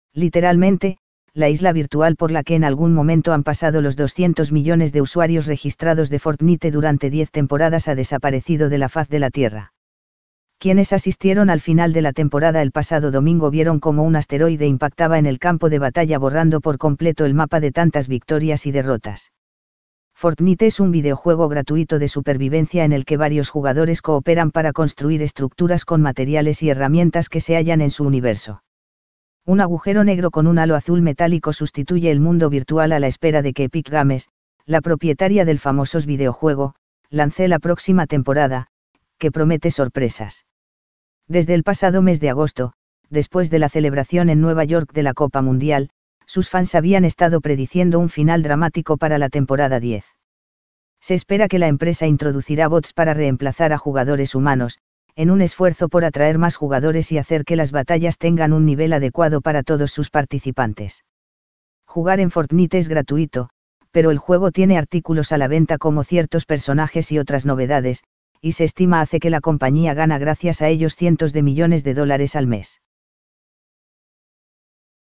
Fortnite: El Impactante Final De La Temporada 10 Del Videojuego Que Mantiene Desconcertados A Sus
Literalmente, la isla virtual por la que en algún momento han pasado los 200 millones de usuarios registrados de Fortnite durante 10 temporadas ha desaparecido de la faz de la tierra.